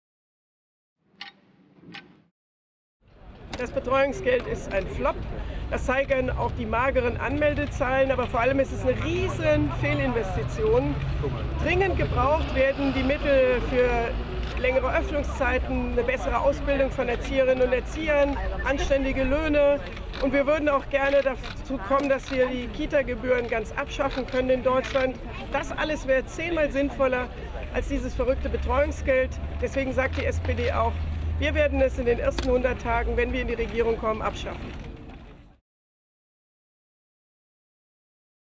Auf die 12 mit Andrea Nahles - noch 52 Tage
Countdown bis zum Wechsel. Andrea Nahles heute von einer Juso-Aktion vorm Kanzleramt: gegen das Betreuungsgeld, für mehr Kitaplätze. "Das Betreuungsgeld ist ein Flop und eine riesige Fehlinvestition", beklagt Nahles.